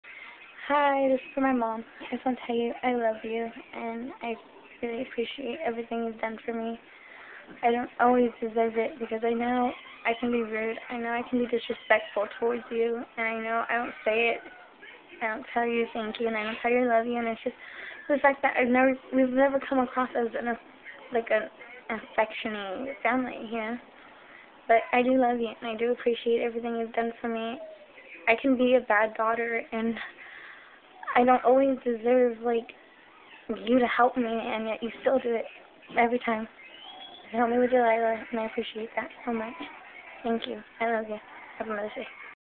For my madre!
I love my mom so much n it sucks cuz I know I come across as such a rude and disrespectful daughter. I was seriously watery eyed in this video. I appreciate her so much even though we both have our faults I love that woman and I need to be more respectful n show my love more cuz I'm lucky to have her..I know not everyone has their moms. So I'm grateful I still do. I love you mom!